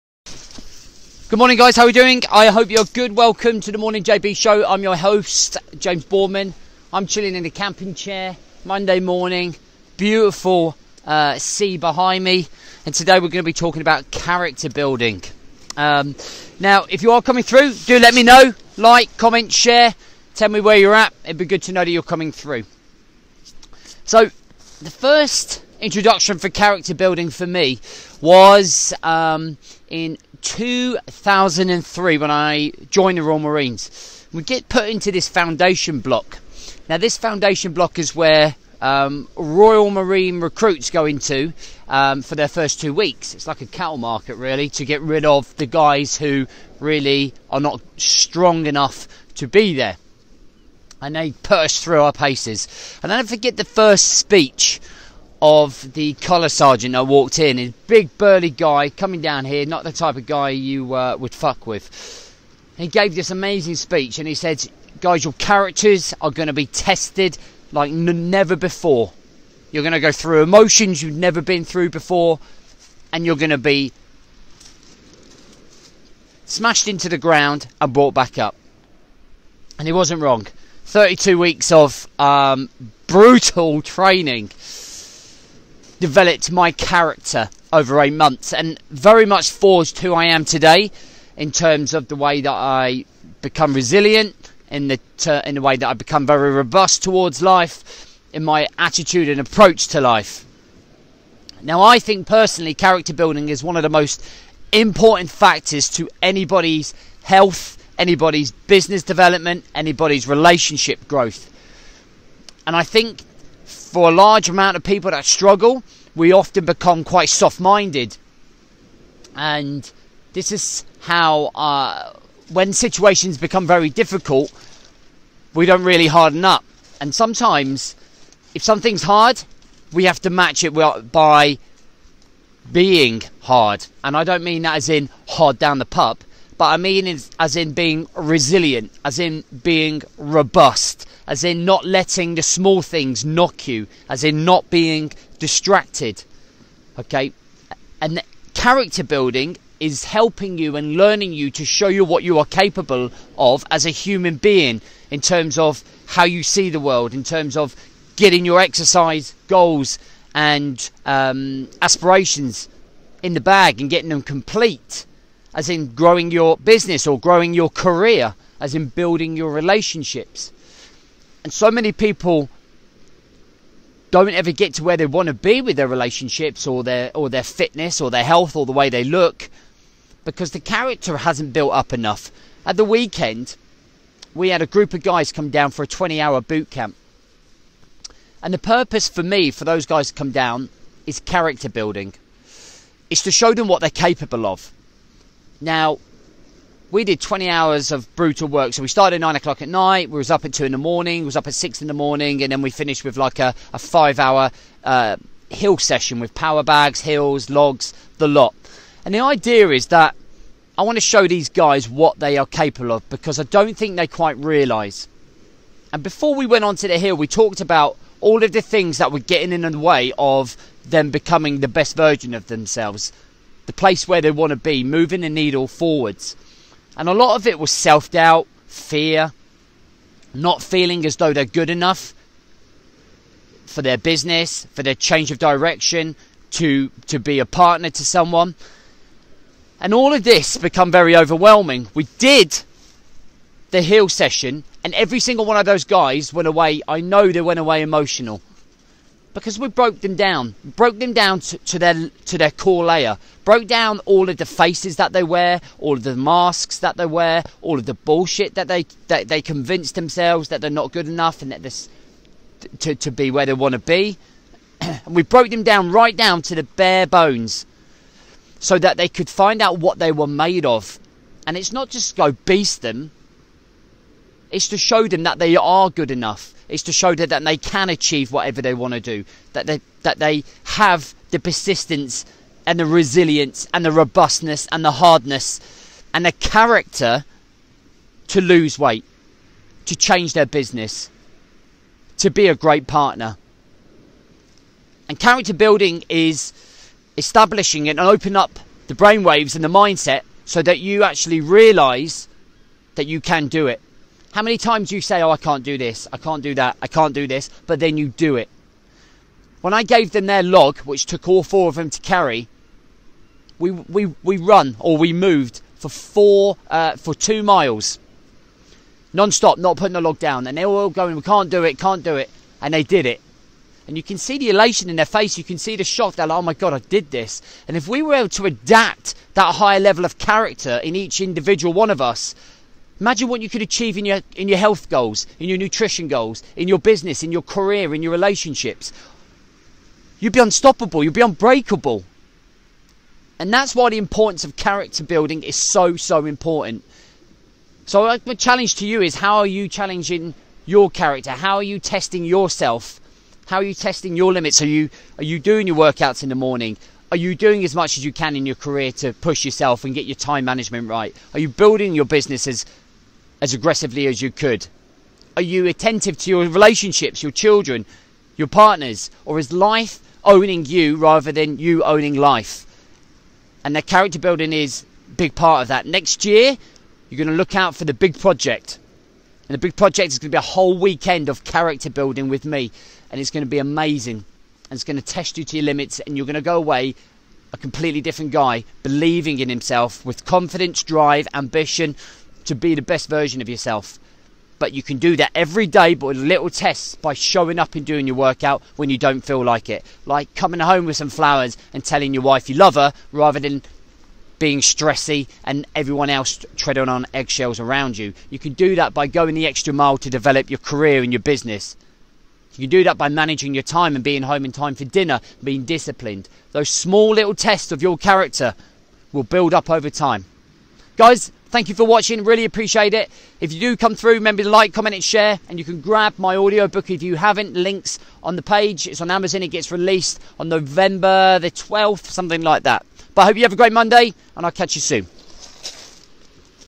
Character Building
🙅🏼‍♂️JB MORNING SHOW 🙅🏼‍♂️

For me character building is an essential part of personal development.

If we plan to grow, we have to build our character to match our ambitions to overcome barriers

Take a listen JB 👇🏼

If you haven’t already you can preorder my AUDIO BOOK

It’s A State Of Mind of Amazon below